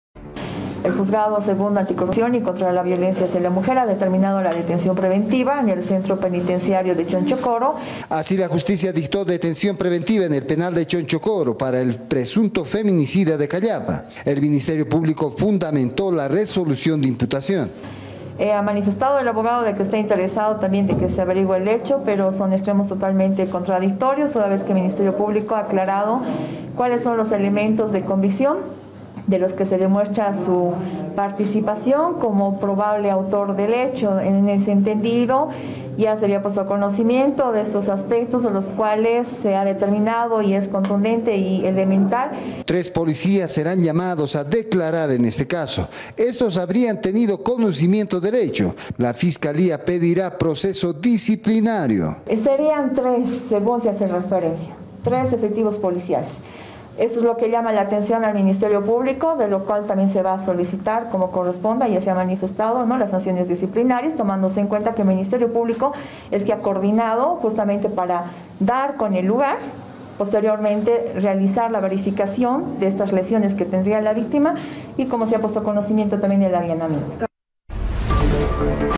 Dictan detención preventiva para el presunto feminicida de Callapa, en el penal de Chonchocoro.
F10Noticias Dictan detención preventiva para el presunto feminicida de Callapa, en el penal de Chonchocoro. Tres policías habrían tenido conocimiento del caso y la Fiscalía pedirá sean sancionados.